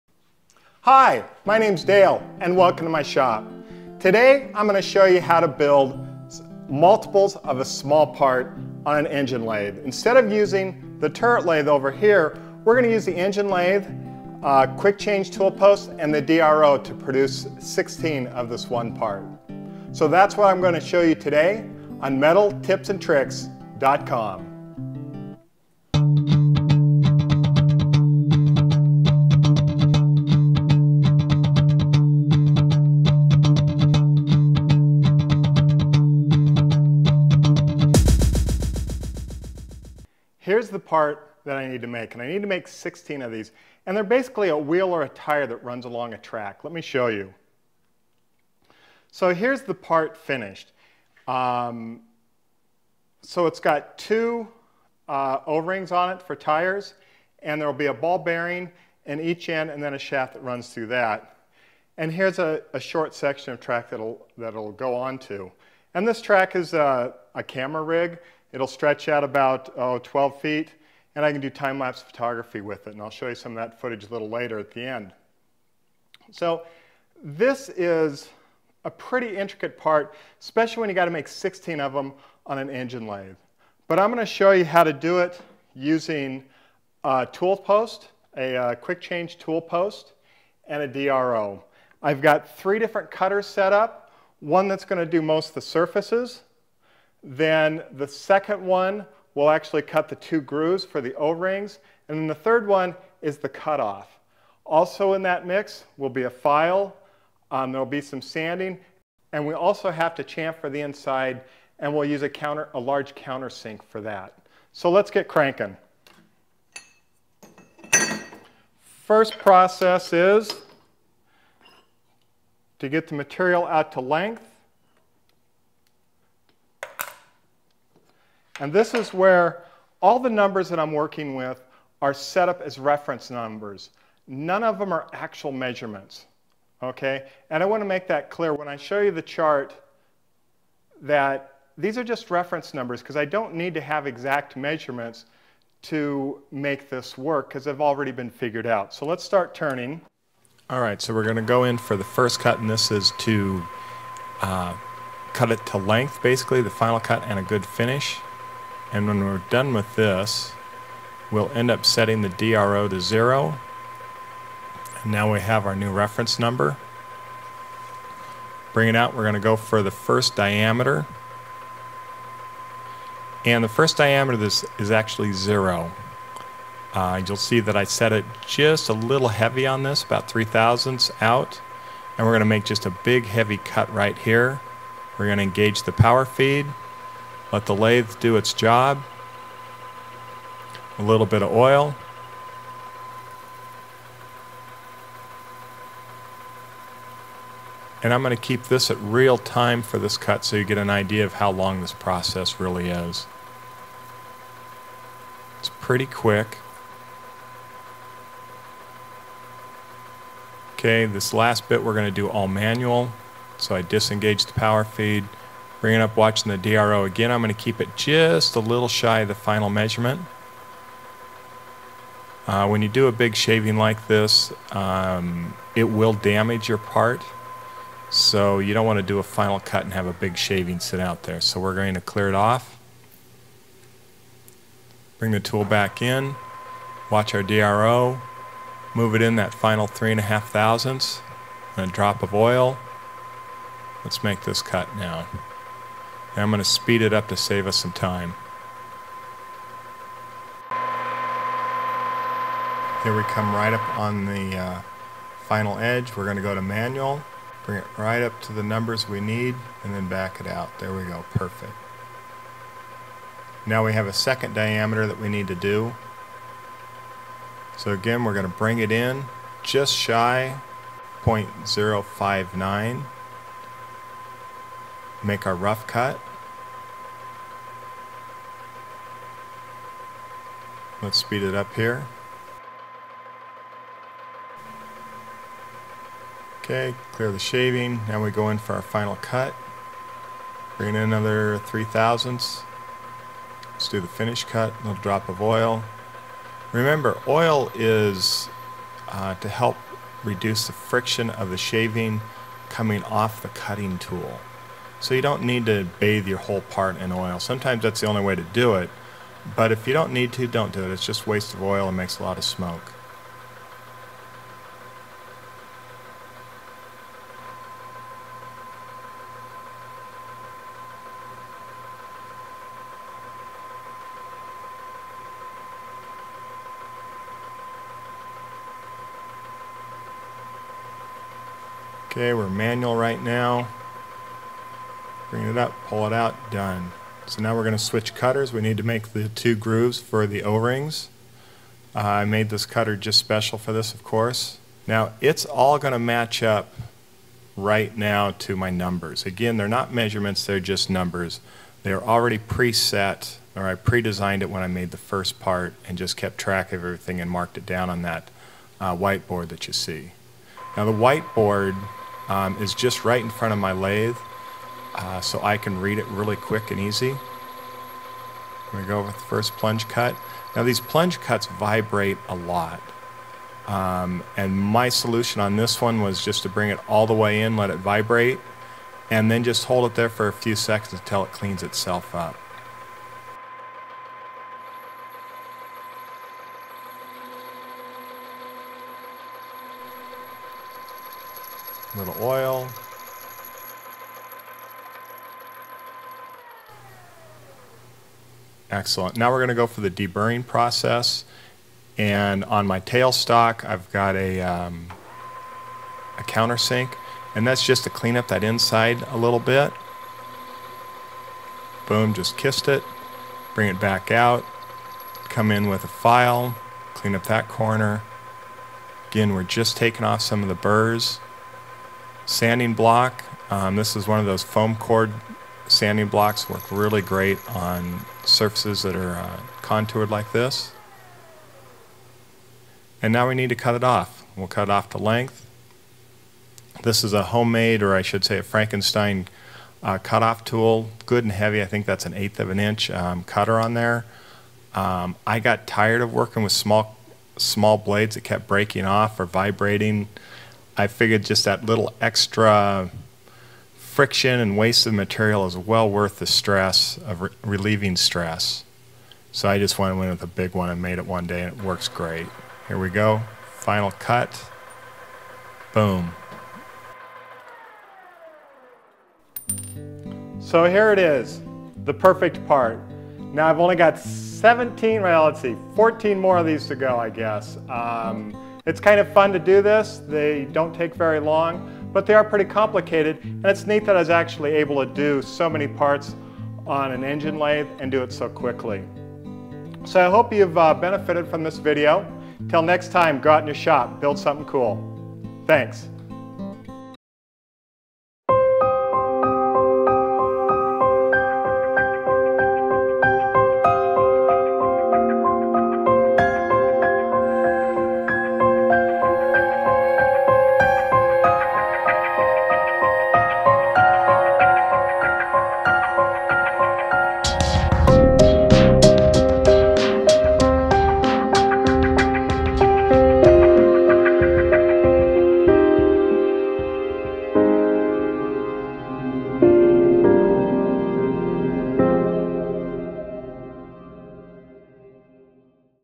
The impossible, done on a metal lathe?
A friend once tried to convince me that it is impossible to make multiples of a part on a manual lathe because it would take too long and you can't hold the tolerances. This video is to prove that if you have a short run of parts, not only is it possible, it can be done quickly and efficiently.